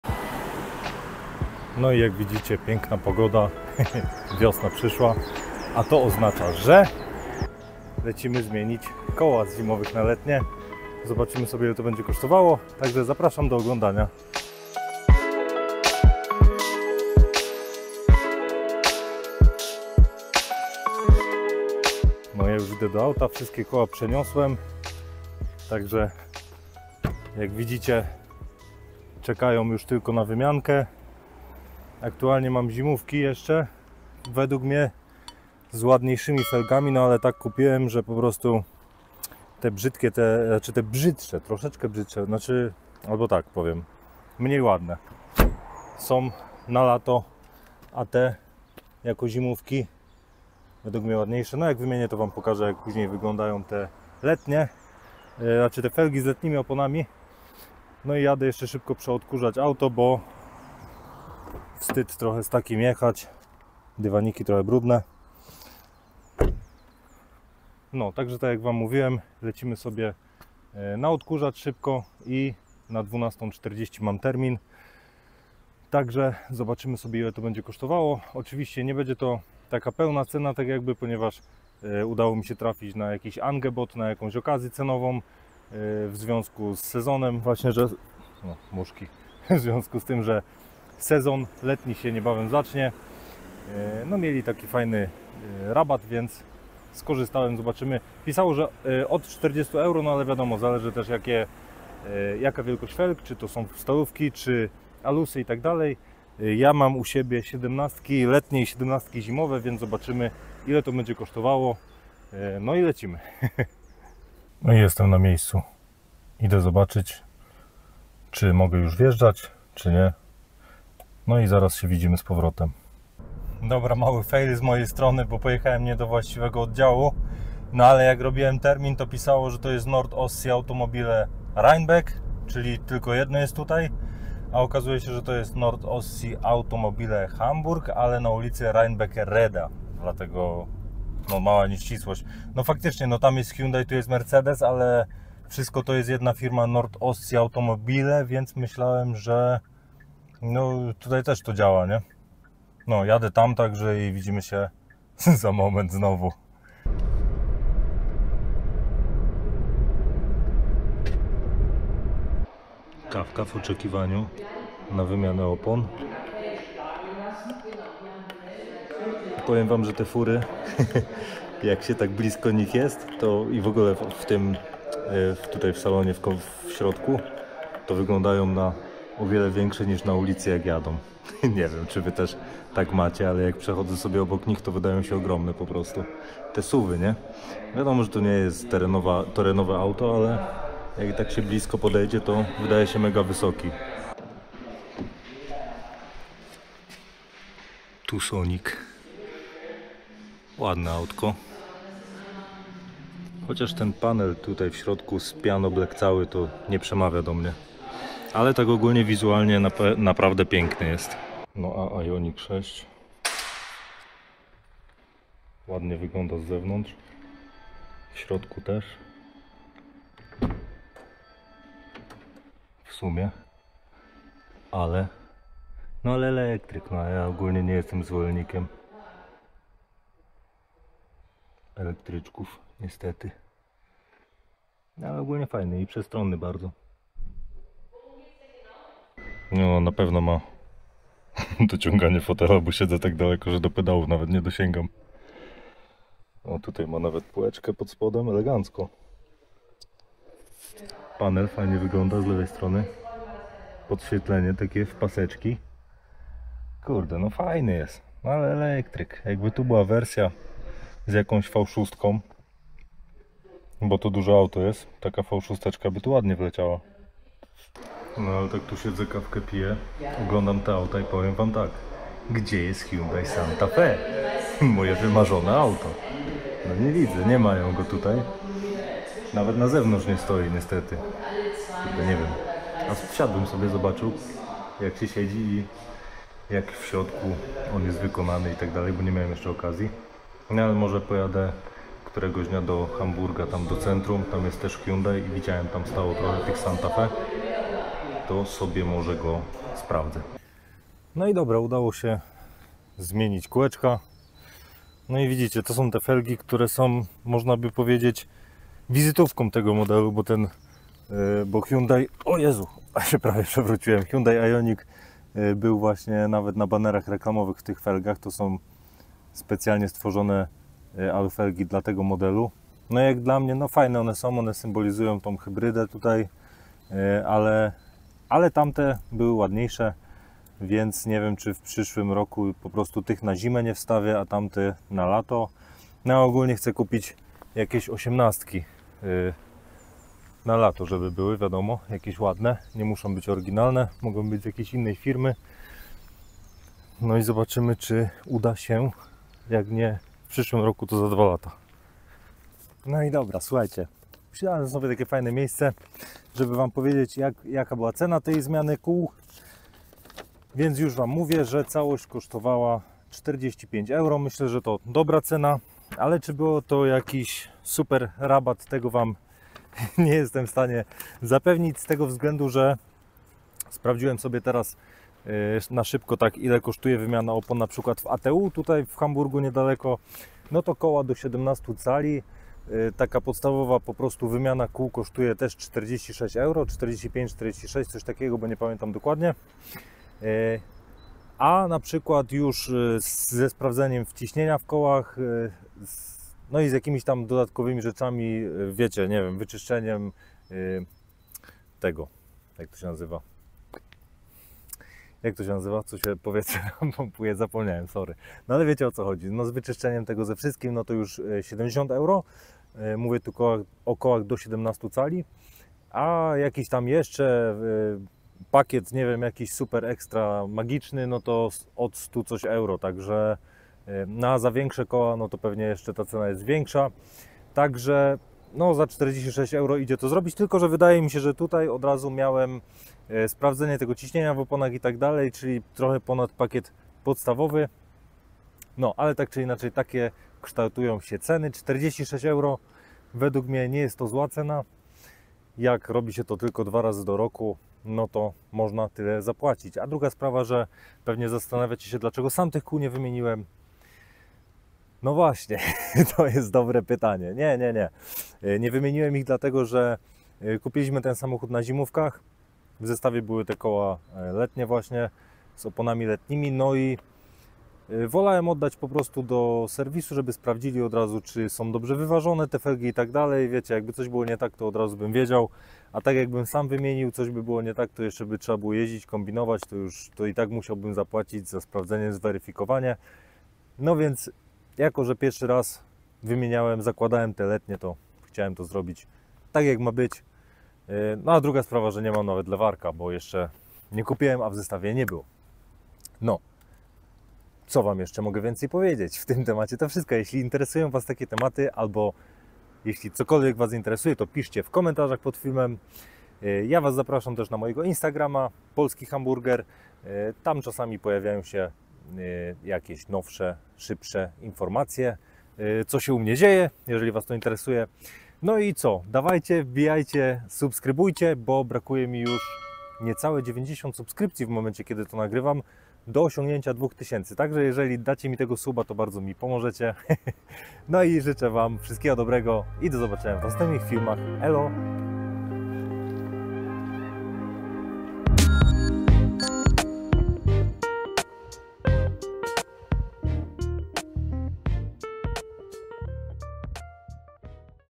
WYMIANA KÓŁ NA LETNIE / NIEMCY
Lecimy do ASO Hyundaia na wymianę kół z zimowych na letnie.
Zobaczymy, ile to będzie kosztowało i czy warto.

💡💡💡Jeśli podobają się Wam moje materiały to zapraszam do udostępniania ich dalej, być może Waszym znajomym też się one spodobają!